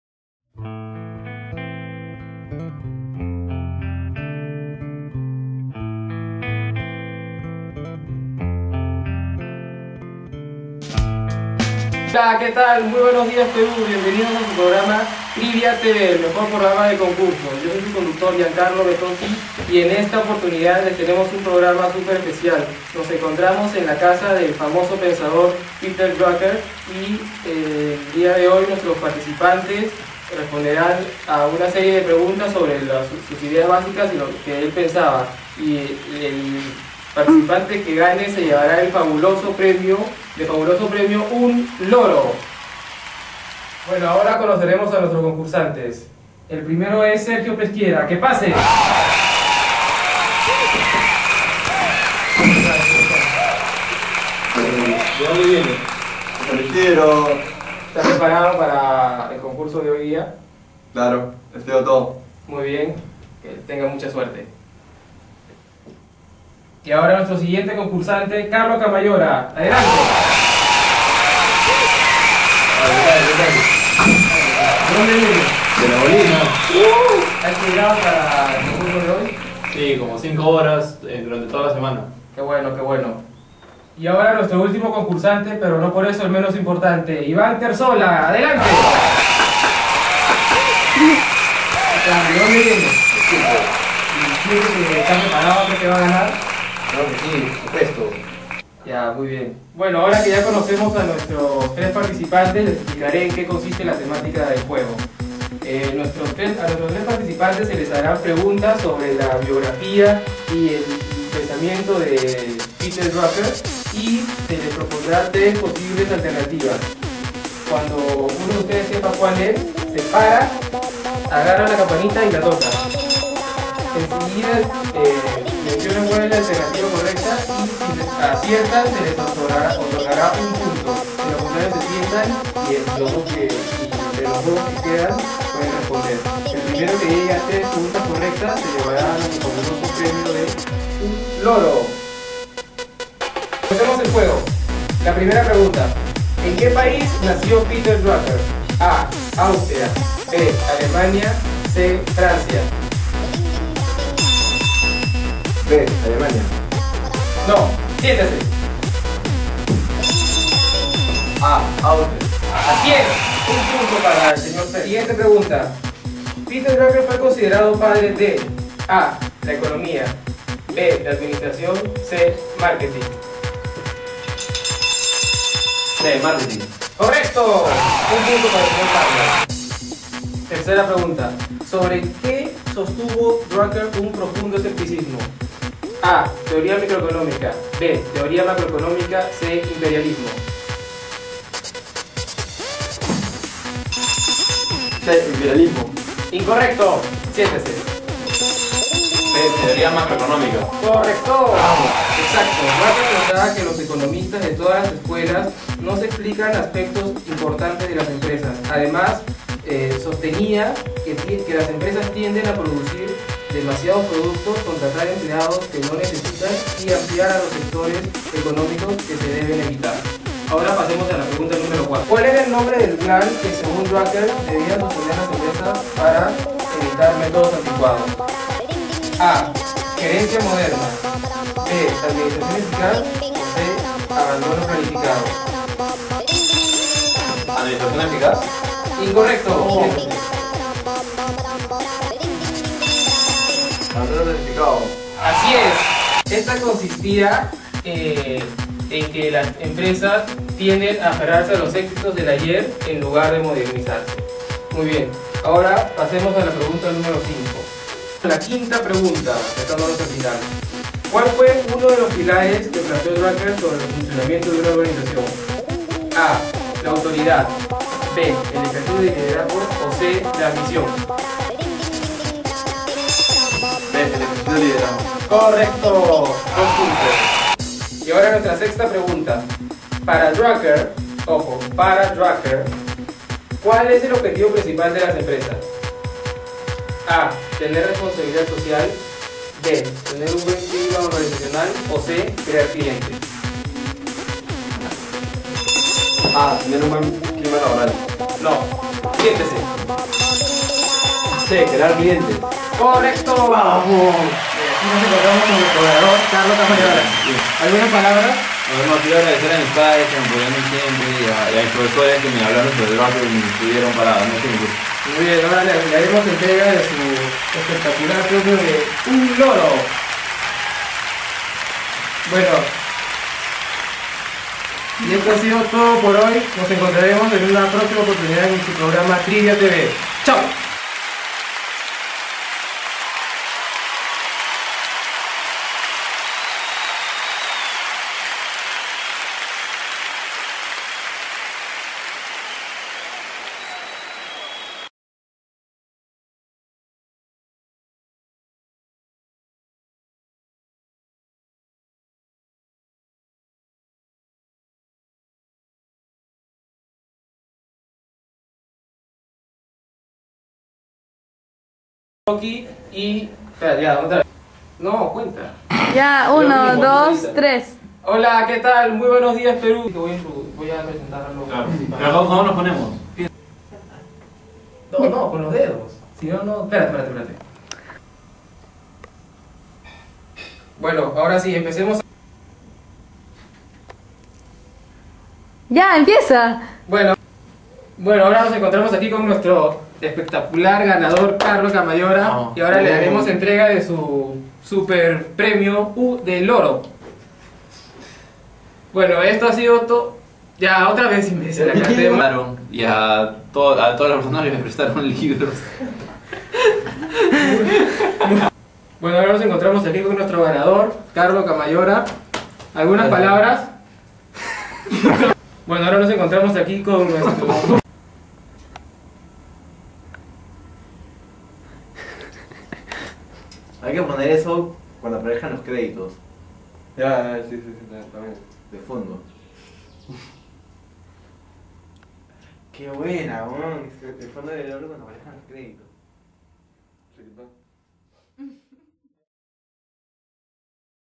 Peter Drucker Trivia Show
A question-answer game about the life and work of Peter Drucker